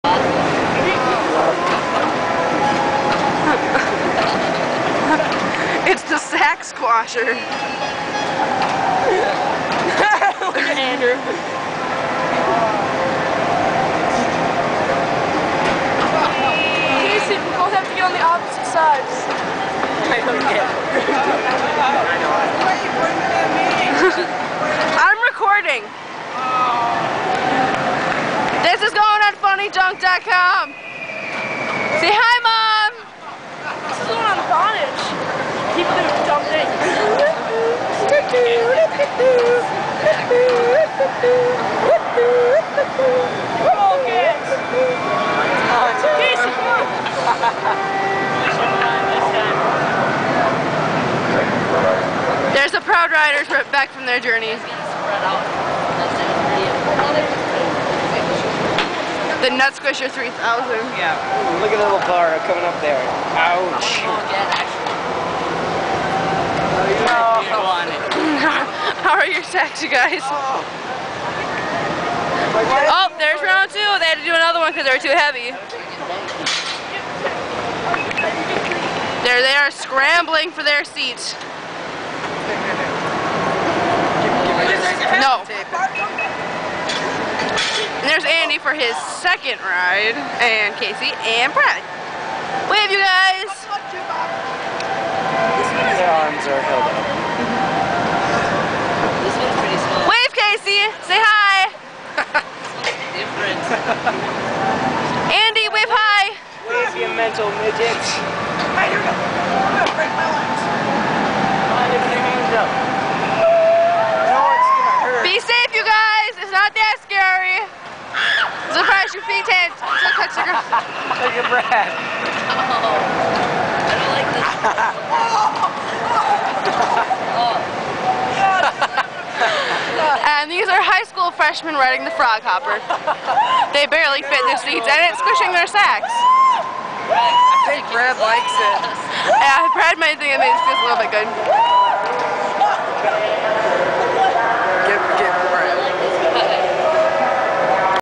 Fun with the Boys
The boys at MOA on "Frog Hopper" getting their nuts squished.